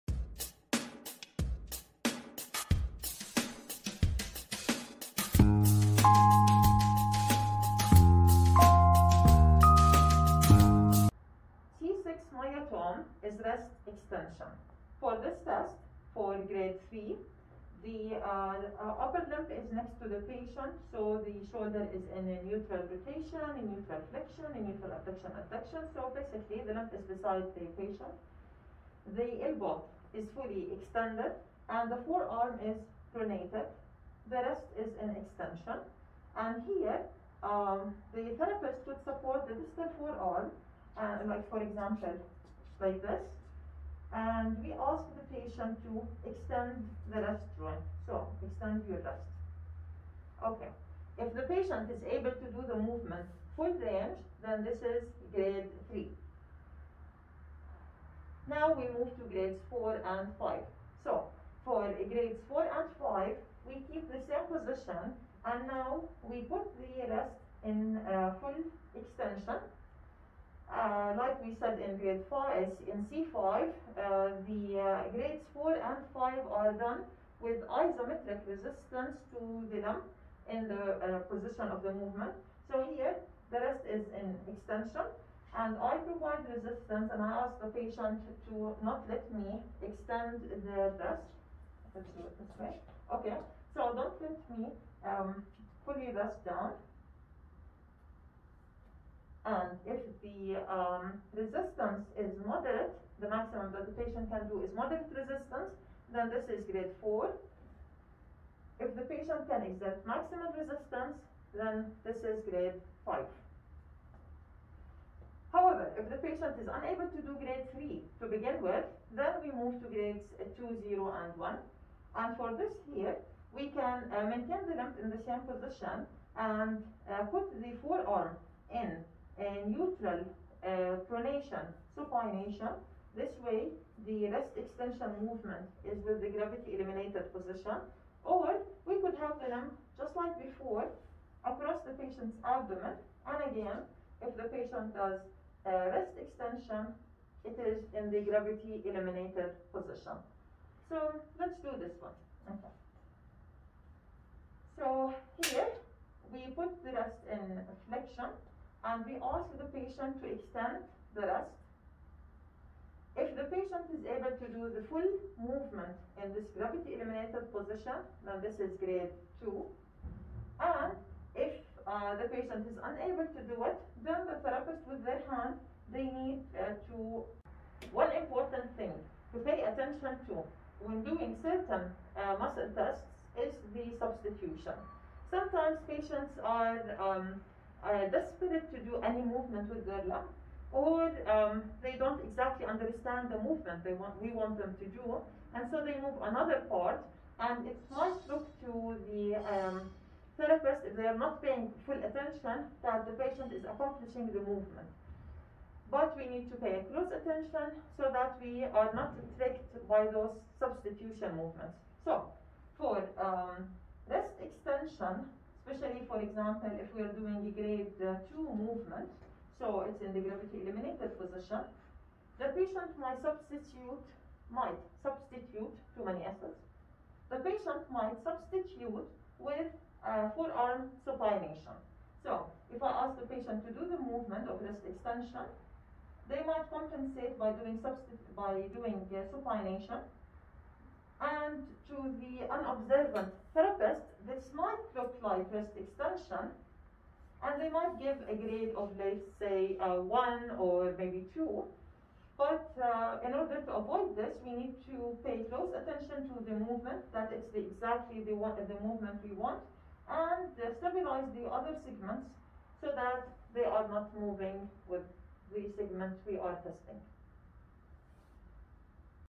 Spinal Cord Injury Motor Exam: C6 Myotome
In this topic, we will cover the management of patients with spinal cord injury (SCI).
This playlist shows the application of motor and sensory exams for patients from supine position. The motor and sensory exams are important to help identify the patient’s neurological level. When performing those tests it is critical to keep the patient in supine position to maintain stabilization of the spine since most patients acquire the SCI due to fractured spine. Because of this reason, testing the myotomes in patients with SCI does not follow the usual technique used with other patients.

Physiotherapy Department - The University of Jordan